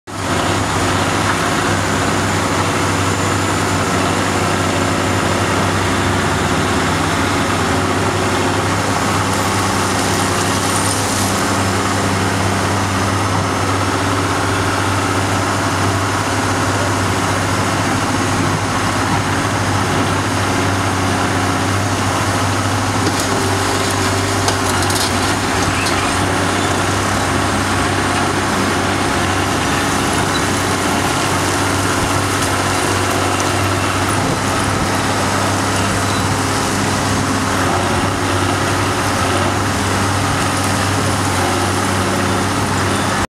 Poclain 115 - Engins passion 2017
A l'occasion du superbe salon Engins Passion à Albon (Drôme 26) dédier au vieux matériel de travaux publics, la Poclain 115 butte  d' E.Moutot effectuais des démonstrations.